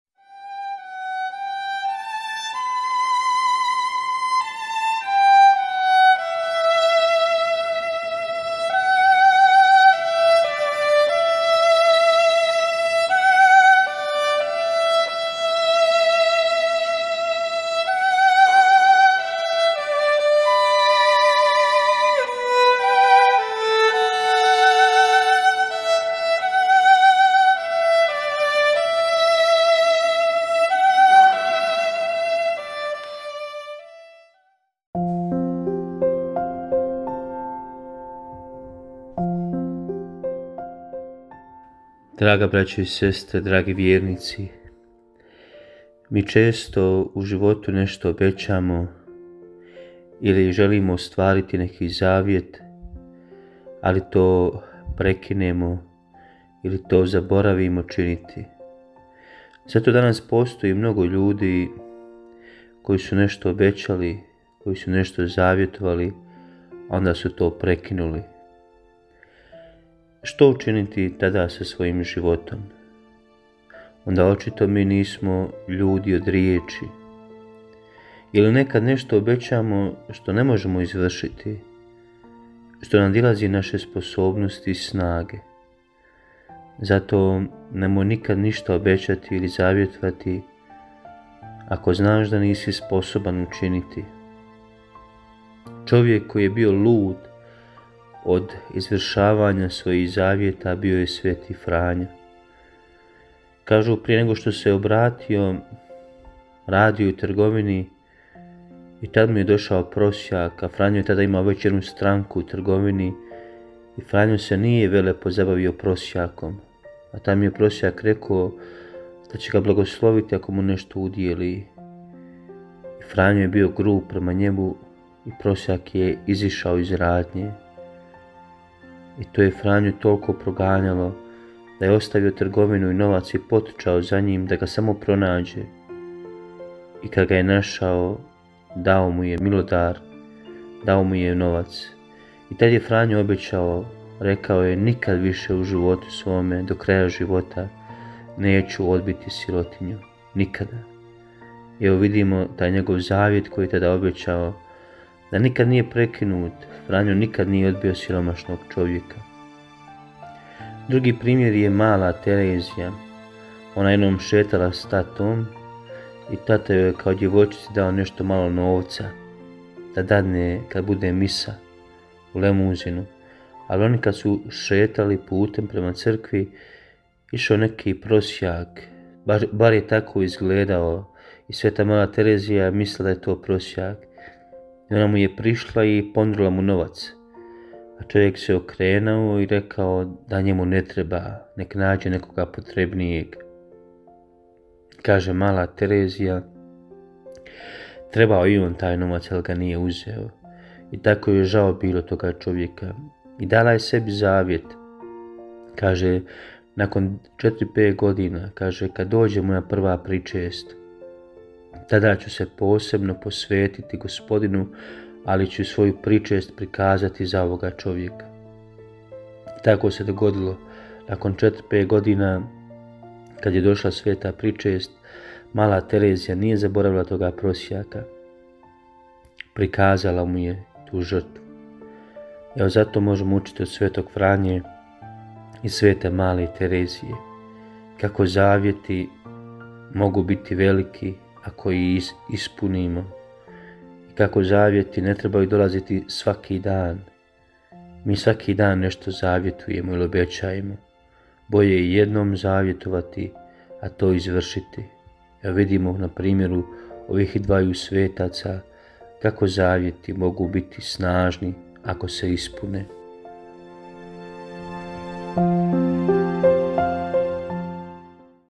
Prekidamo li naše zavjete i obećanja - Fra Goran Azinović - Duhovna meditacija 167.
Duhovne meditacije fra Gorana Azinovića

PODRŽITE NAS VAŠIM ČLANSTVOM!